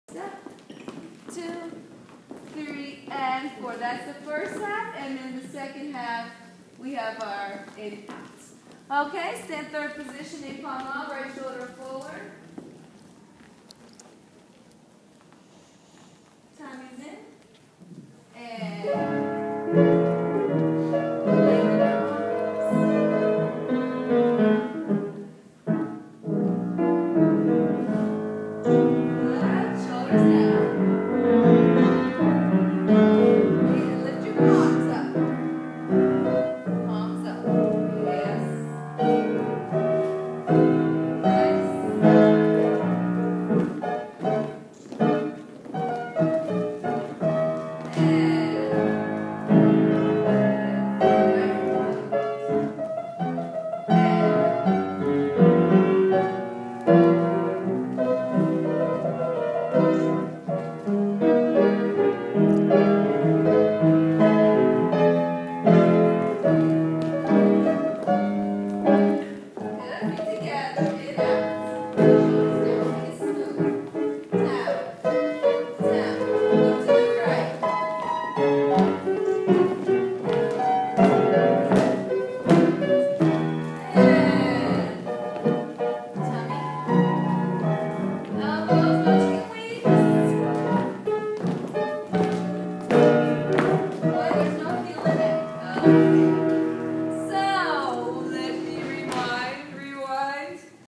Ballet Character Dance Class-JKO School of American Ballet Theatre
Alexis (10 y.o.) at ABT (2012)